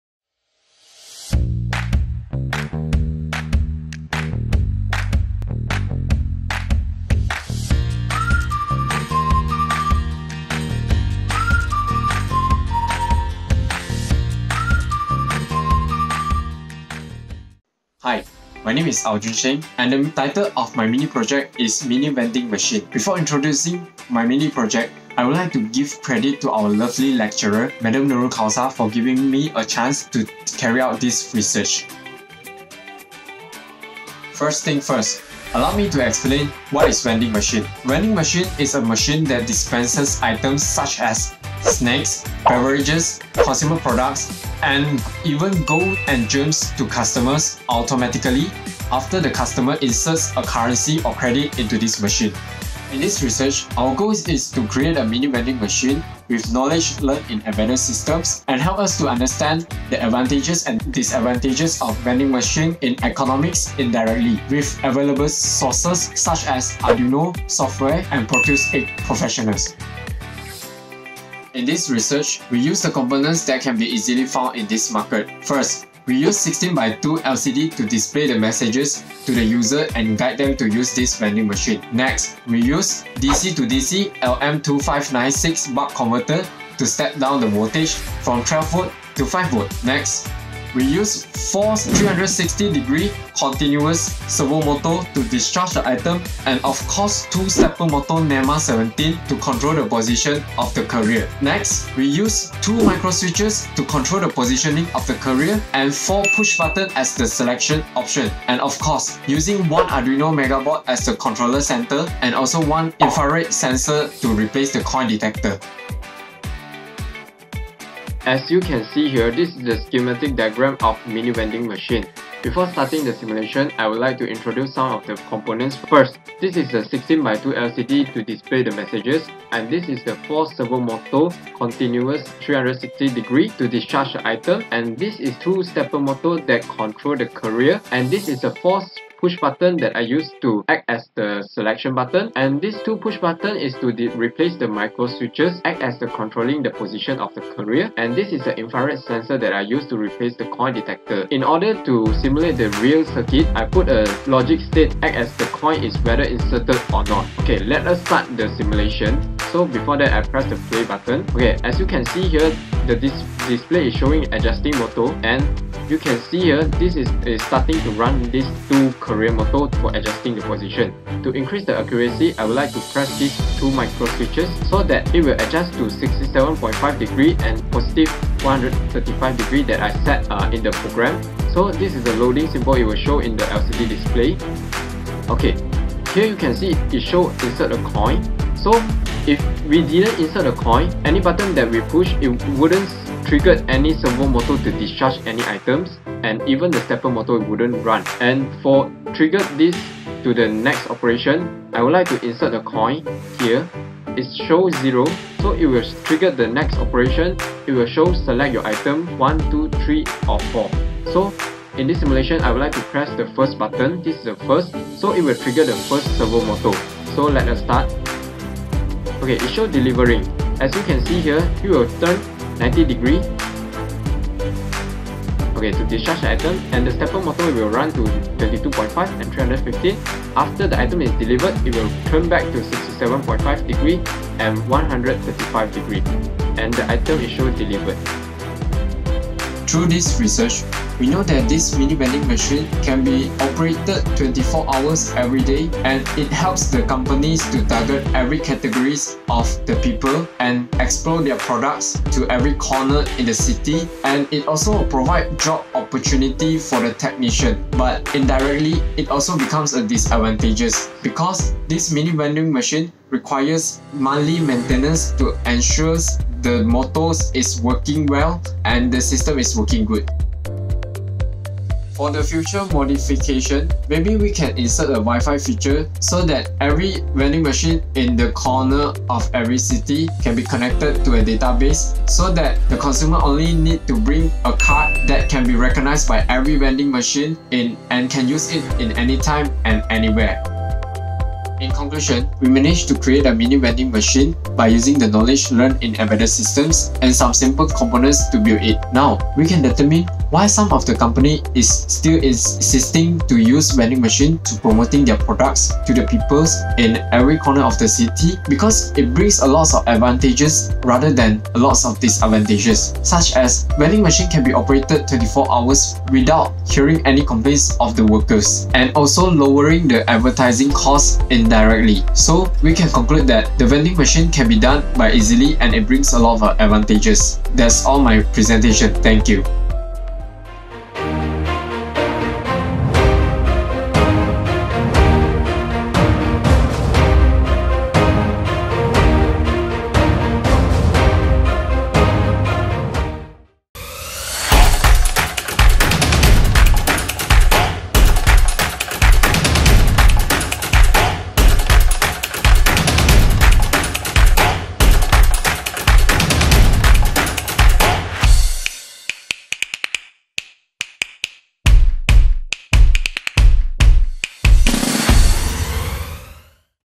Mini Vending Machine
Vending machines brings a lot of advantages than disadvantages. Thus, our goal is to create a mini vending machine with knowledge learnt in Embedded System and study why some company still using Vending machine to promote their products
Name : Ow Jun Sheng
Section: 2/1
Course: 2BEEA
Lecturer: Madam Nurul Kausar Binti Ab Majid
Software used:
Arduino - Coding
Proteus 8 Professional - Draw Schematic diagram
Adobe Premiere Pro 2018 - Editing Video